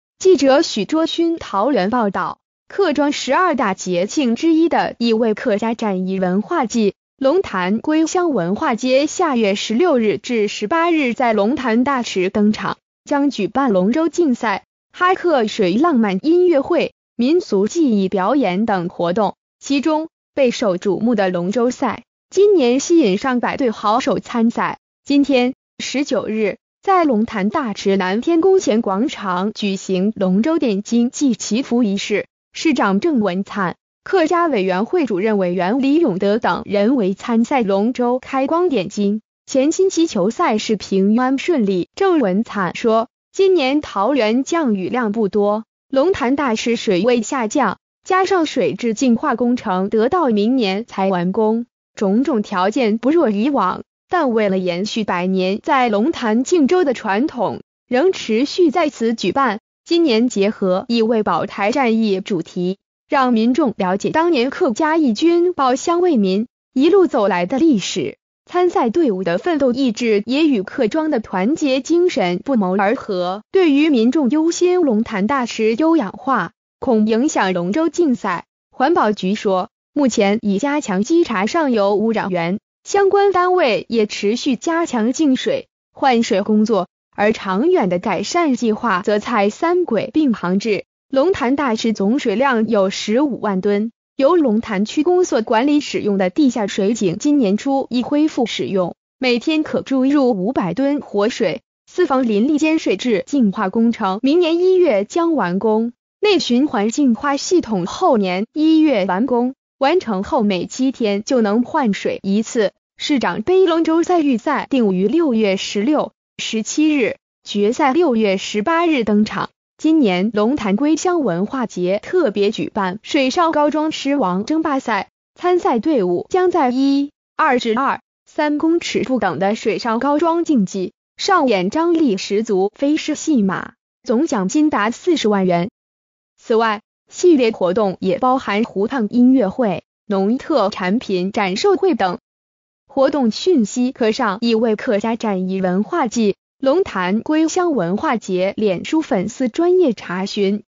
龍潭大池優養化 鄭文燦：龍舟賽照辦延續傳統
〔記者許倬勛／桃園報導〕客庄十二大節慶之一的「乙未客家戰役文化季．龍潭歸鄉文化節」下月16日至18日在龍潭大池登場，將舉辦龍舟競賽、哈客水浪漫音樂會、民俗技藝表演等活動，其中，備受矚目的龍舟賽，今年吸引上百隊好手參賽，今天（19日）在龍潭大池南天宮前廣場舉行「龍舟點睛暨祈福儀式」，市長鄭文燦、客家委員會主任委員李永得等人為參賽龍舟開光點睛，虔心祈求賽事平安順利。

發表評論評論如下。